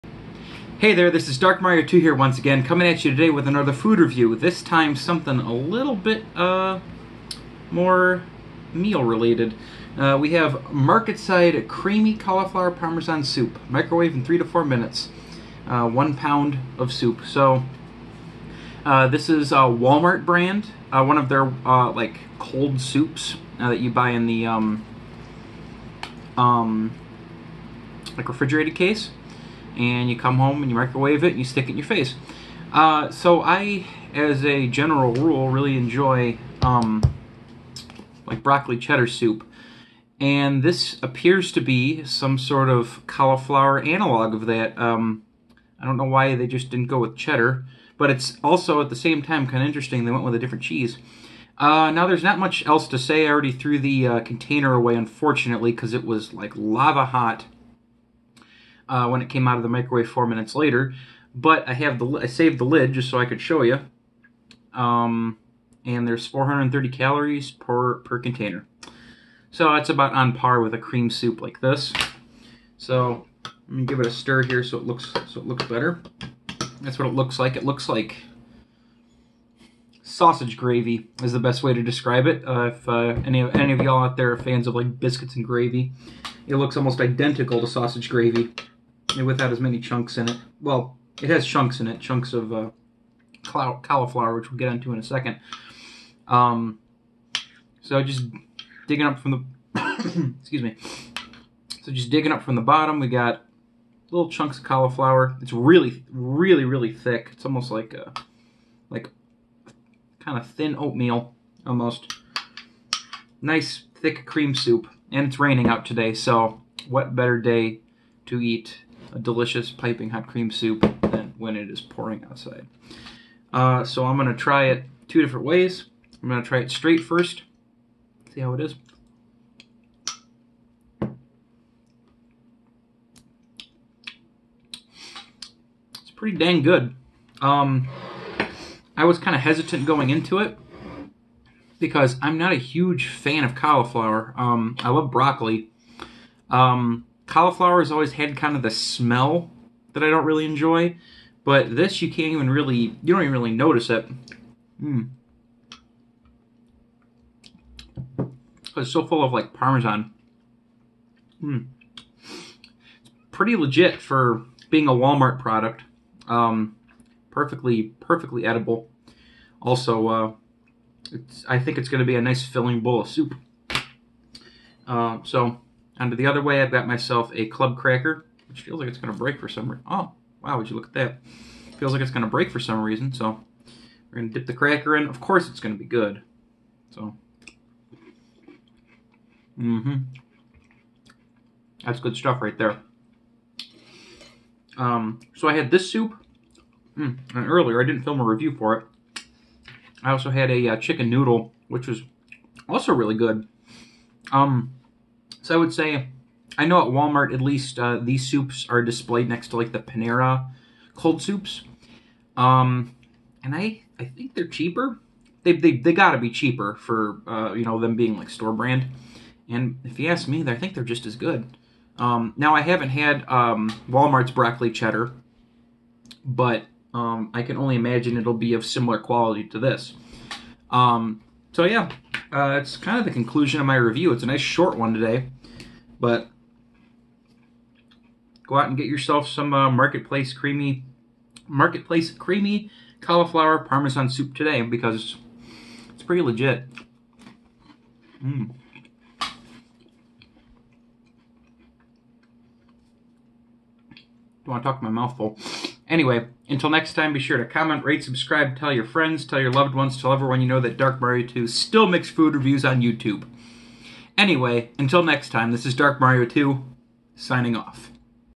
Walmart Marketside Creamy Cauliflower Parmesan Soup Review
Eat your heart out Panera, the stuff is pretty legit.